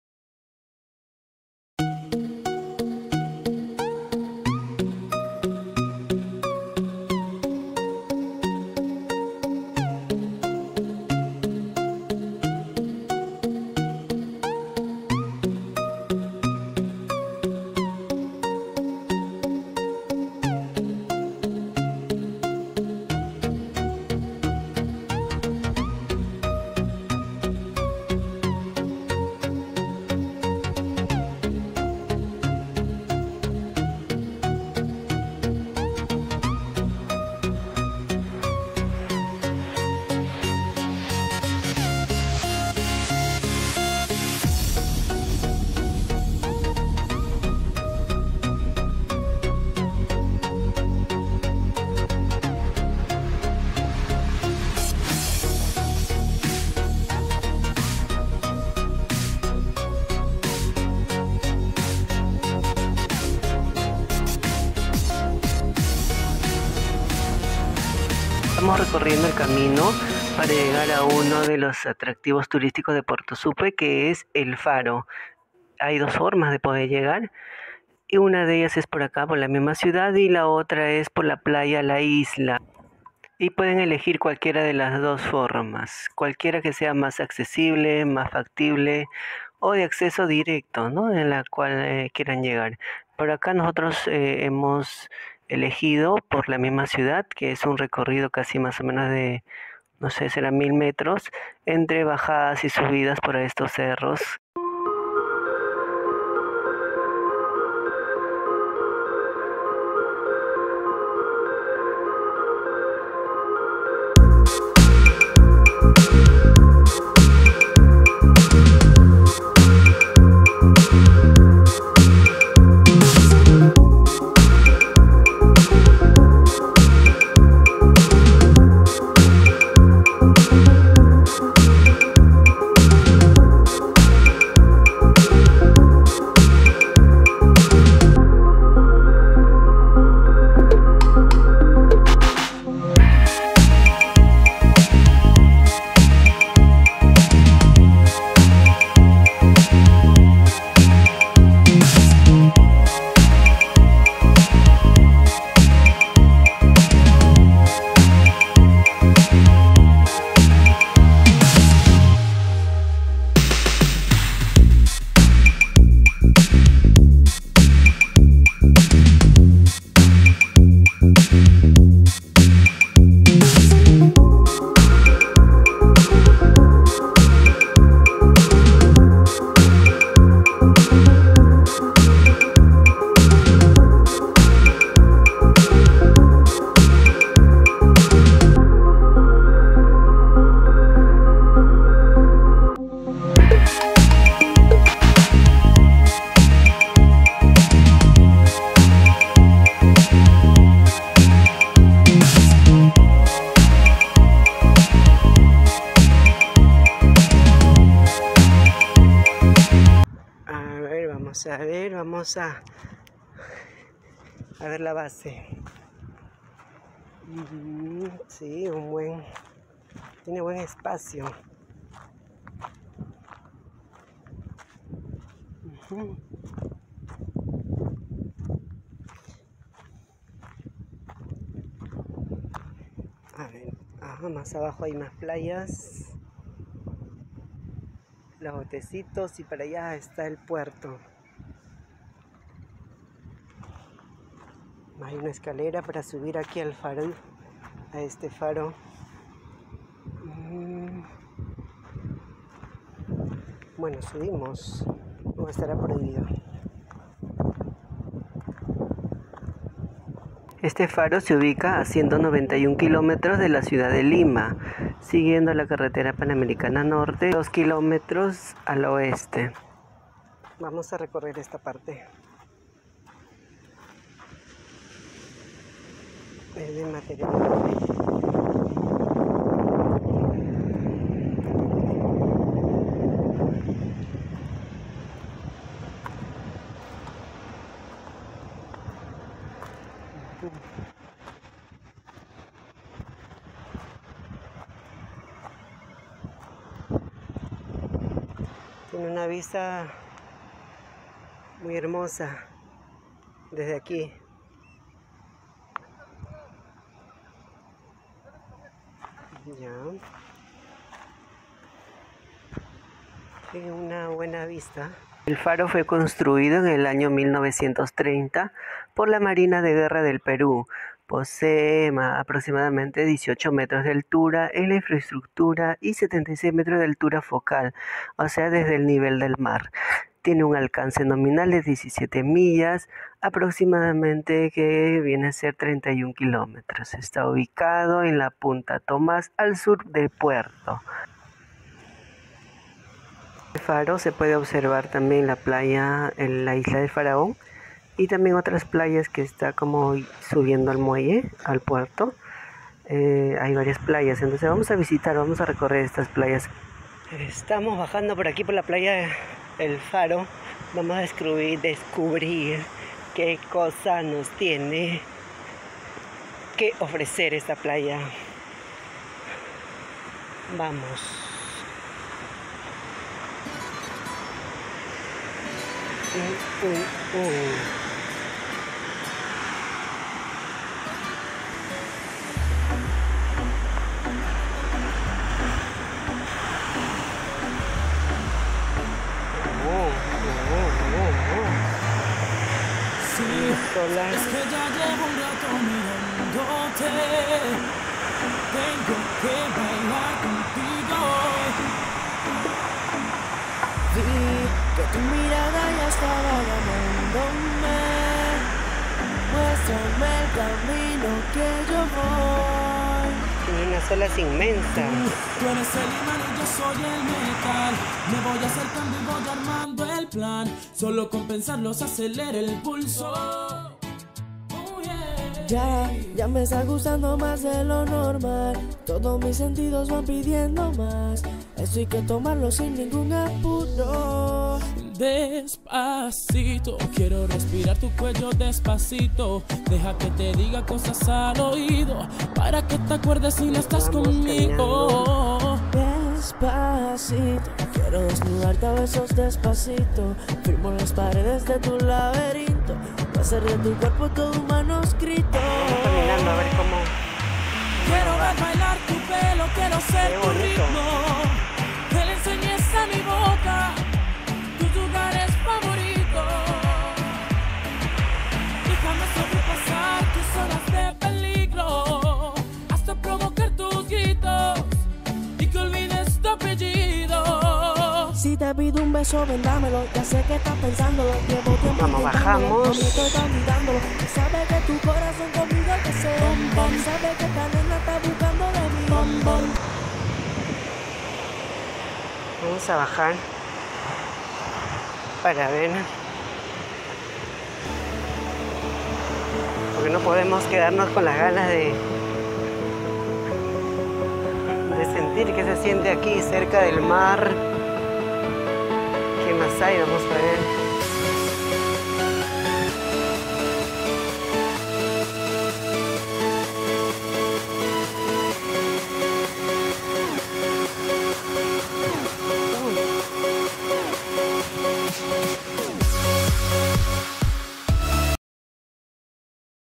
ElFaro en SupePuerto
ElFaro es uno de los atractivos Turisticos que tiene SupePuerto, pose 18 metros de altura, magnifico mirador de la Playa La Isla